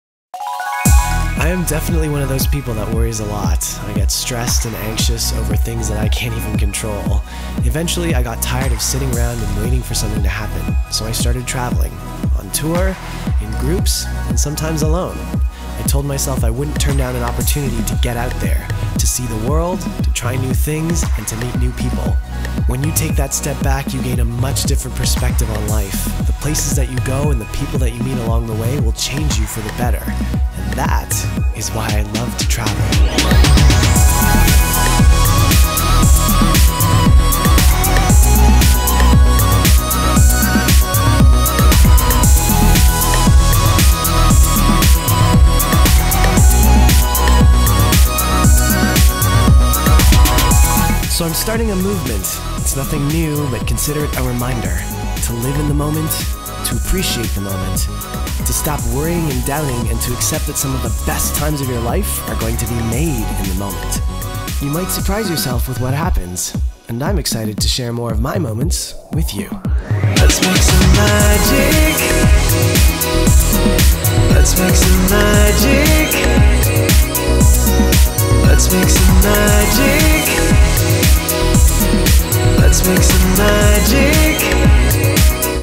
Why I Travel: Made In The Moment | gunnarolla
Hi, I'm gunnarolla - a music and video producer who travels the world. This is the story of MadeInTheMoment, a hashtag and movement that encapsulates my life philosophy and approach to content.

As you may have noticed, I've been travelling a lot lately - not just for my job, but for myself. I was lost for a while, but travel has really helped me remember what is important, and how to be happy. I'm excited to share so many of the moments I've made over the past year with you. ‪MadeInTheMoment